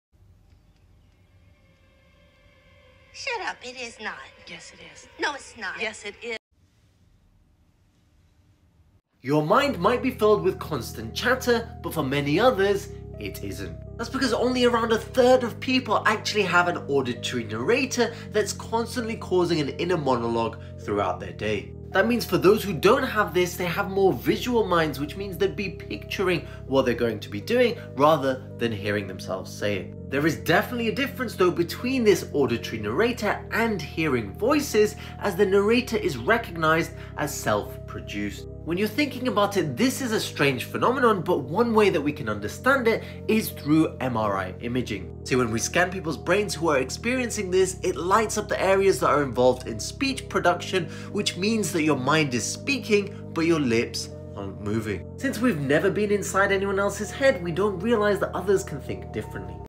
“I Thought Everyone Had That”
Not everyone experiences their thoughts as an internal auditory narrator, and this difference is a fascinating aspect of human cognition. For some, thoughts take the form of a continuous internal monologue—a voice in their mind that narrates their experiences, reflects on decisions, or plans for the future. This "mental narrator" can feel like an ever-present companion, articulating their thoughts in words throughout the day.

However, many people don’t have this auditory inner voice. Instead, their thoughts might manifest as abstract concepts, images, or feelings that don’t require verbalization. These individuals process the world differently, often relying on visualization or emotional intuition rather than spoken words in their mind.

Neither experience is better or worse; they simply reflect the diverse ways humans process their inner worlds. This variation can influence how people approach problem-solving, creativity, and communication. Understanding these differences highlights how unique and multifaceted human thought truly is.